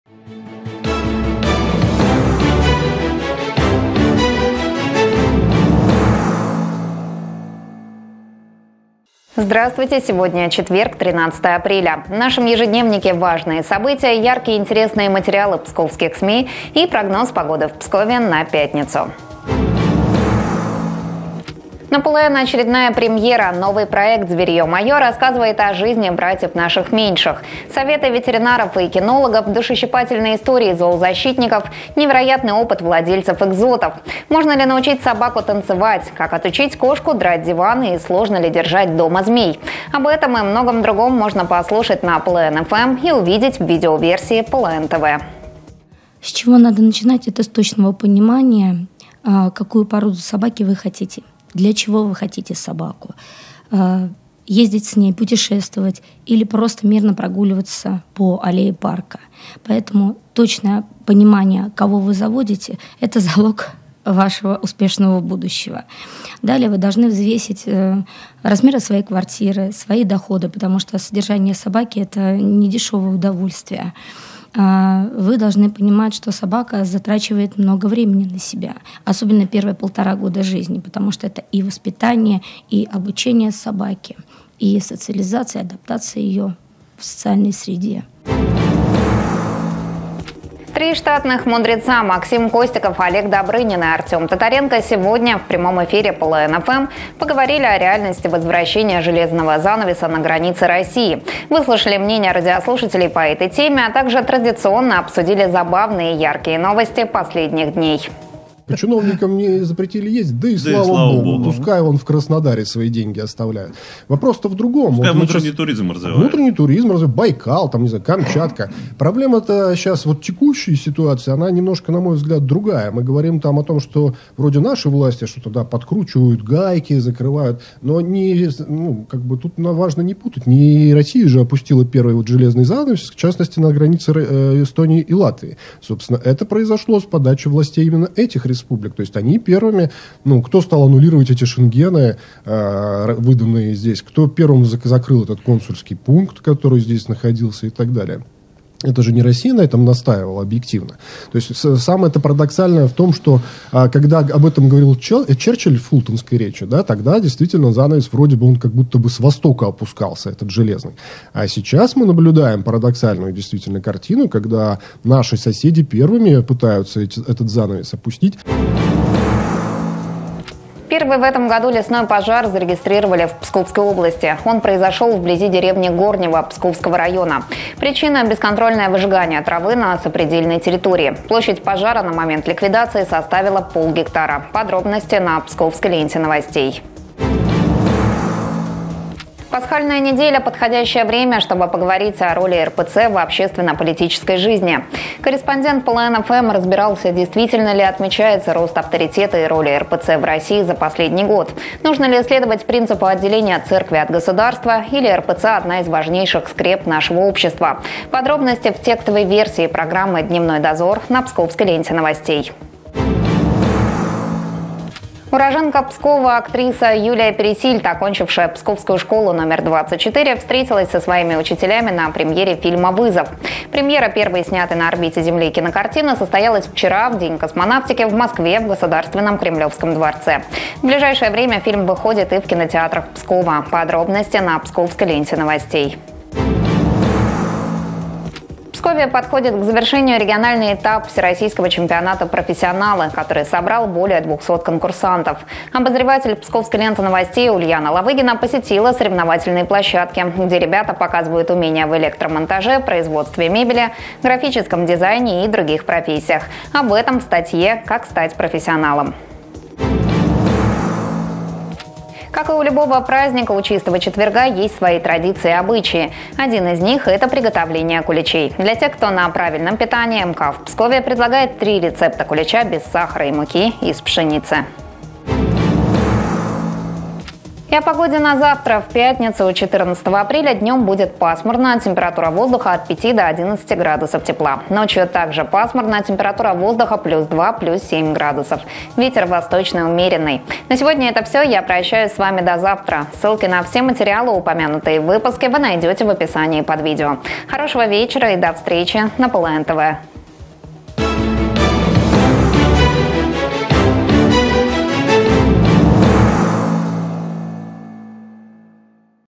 Важные новости Псковской области 13 апреля 2023 г.
«ЕЖЕДНЕВНИК» за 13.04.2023 г. Важные события, интересные и яркие материалы псковских СМИ и прогноз погоды в Пскове на среду.

«Три мудреца»: возвращение «железного занавеса» — миф или реальность?

Первый в этом году лесной пожар зарегистрировали в Псковской области

Юлия Пересильд встретилась с псковскими педагогами на премьере фильма «Вызов».

Как стать профессионалом?

Максимум пользы: три рецепта пасхального кулича для тех, кто на правильном питании

Ведущая: Александра ИВАНОВА

Видео ПЛН-ТВ